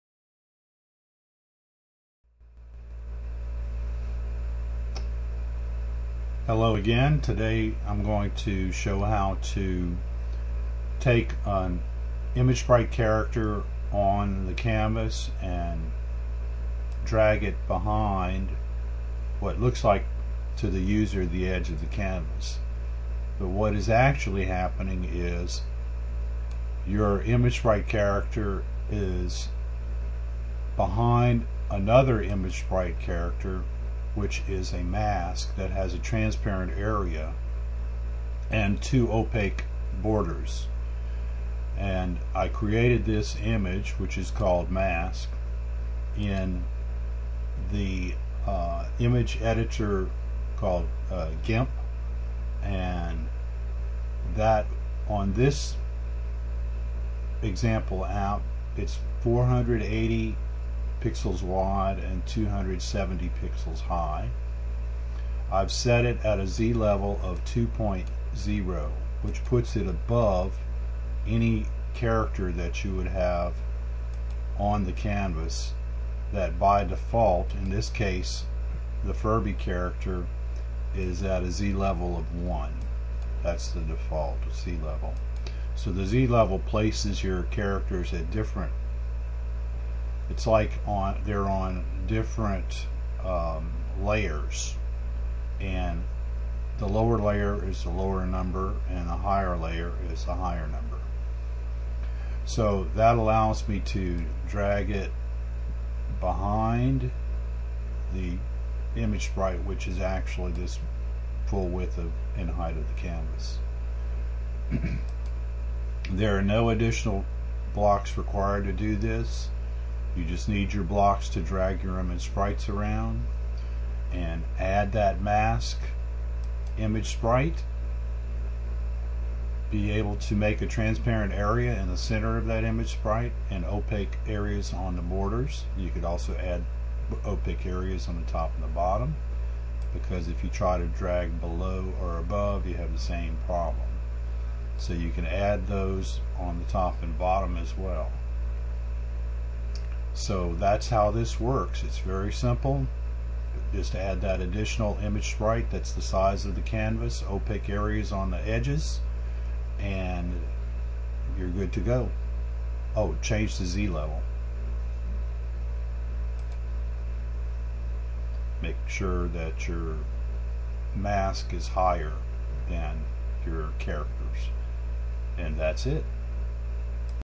App Inventor - Canvas Masks
We cannot drag ImageSprites on or off a Canvas in App Inventor.
But we can make an image mask which contains opaque borders with a central transparent area that is set to the width and height of the Canvas and place that inside another mask ImageSprite.
Placing the mask ImageSprite at a higher Z-level than the other ImageSprites allows them to be dragged or moved behind the opaque borders of the mask ImageSprite.
This video demonstrated how to do this.